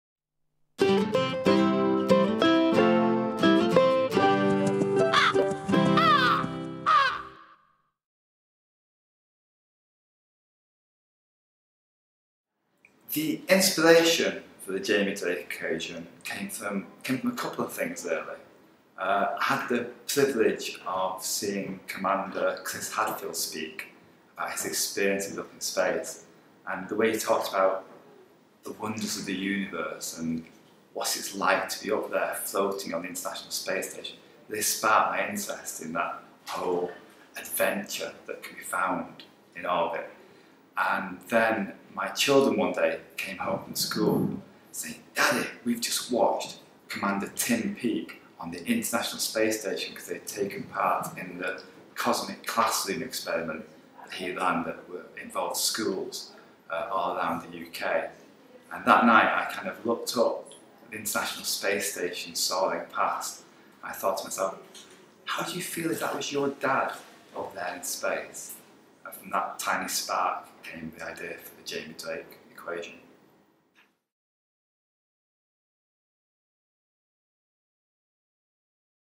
Christopher Edge Q&A: The inspiration for The Jamie Drake Equation
Author Christopher Edge explains the inspiration for The Jamie Drake Equation.